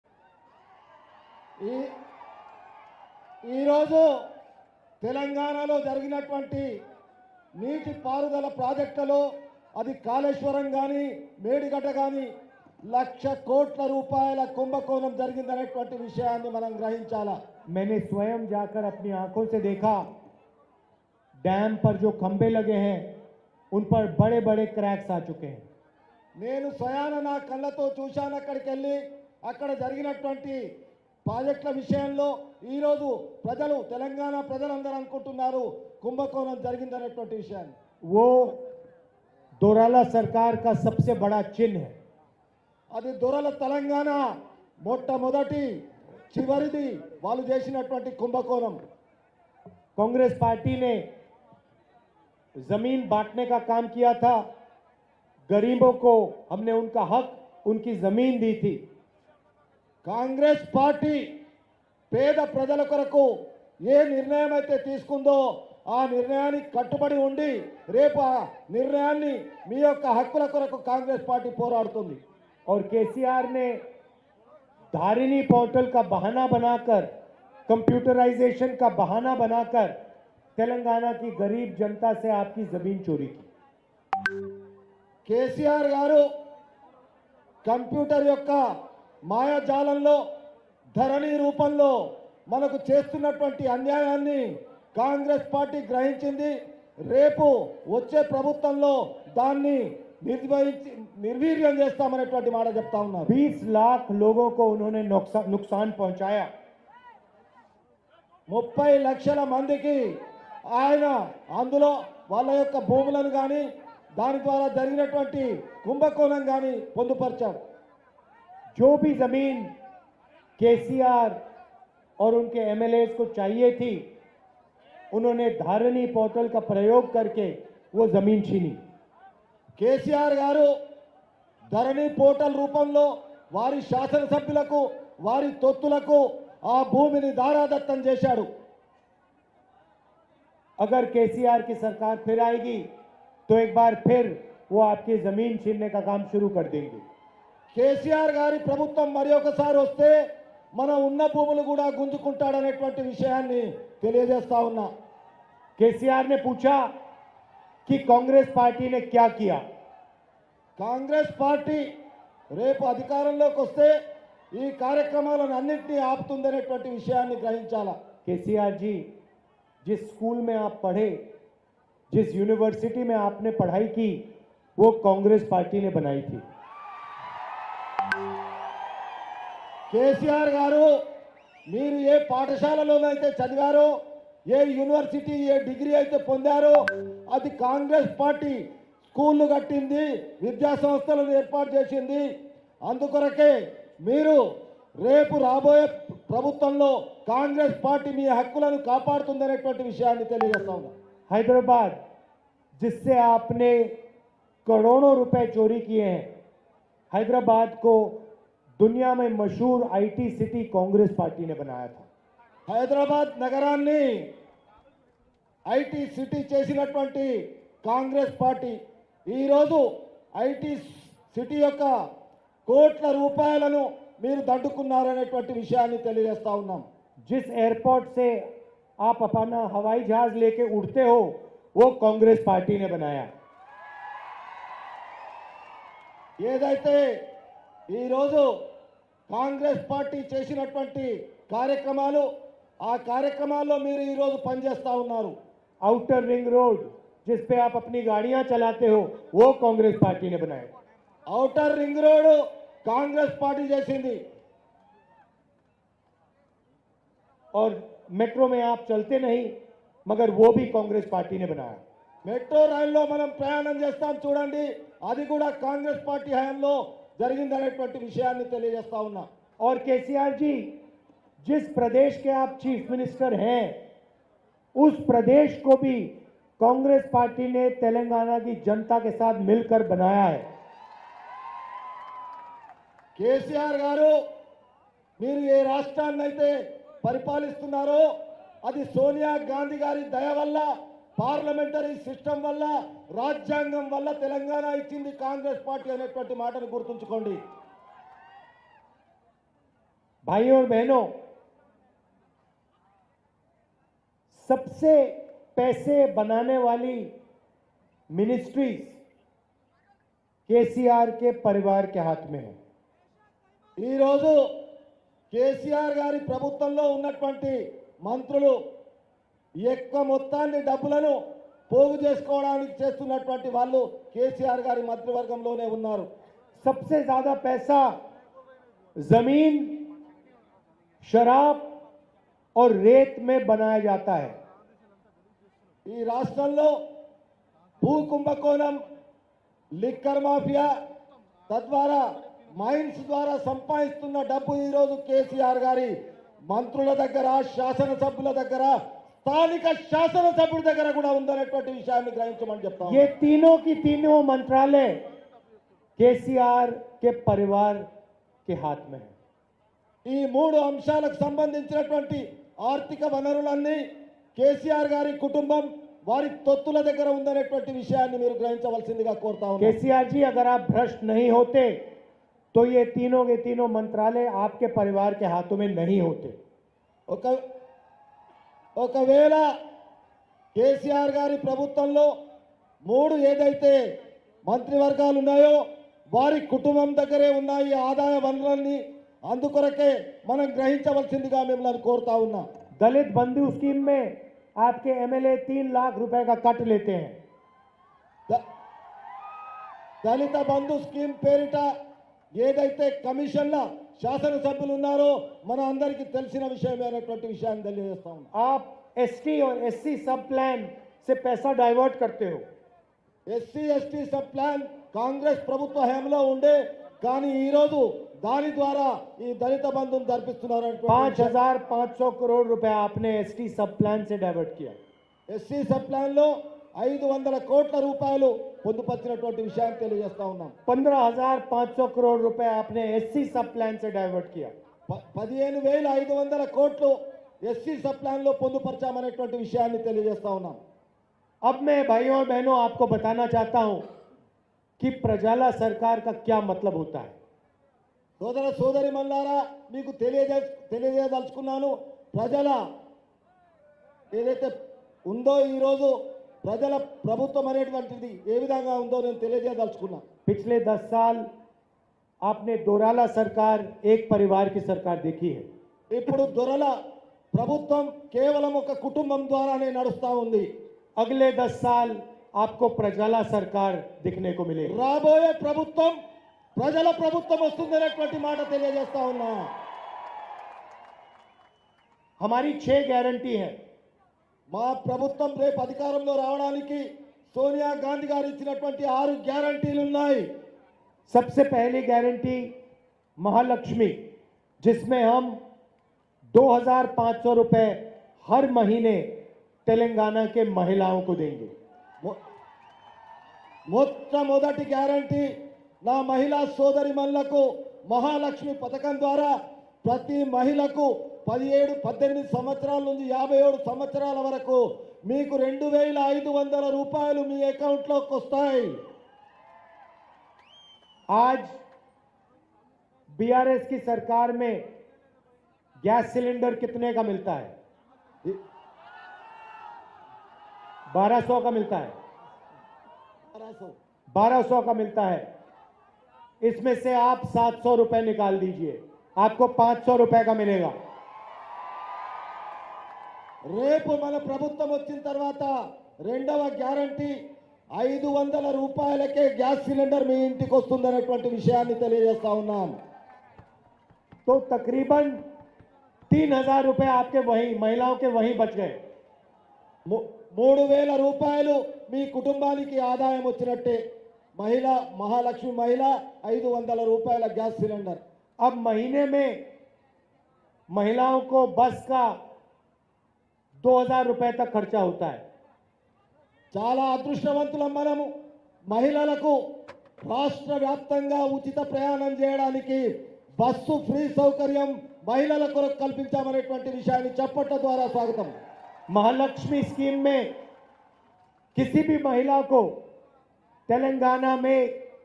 ధరణి ద్వారా భూములను మింగేస్తున్న భూ బకాసురుడు కేసీఆర్..| Rahul Gandhi Speech In Bhodan | KCR| ALO TV
Rahul Gandhi LIVE : Rahul Gandhi Public Meeting At Bhodan LIVE | Revanth Reddy | Congress | ALO TV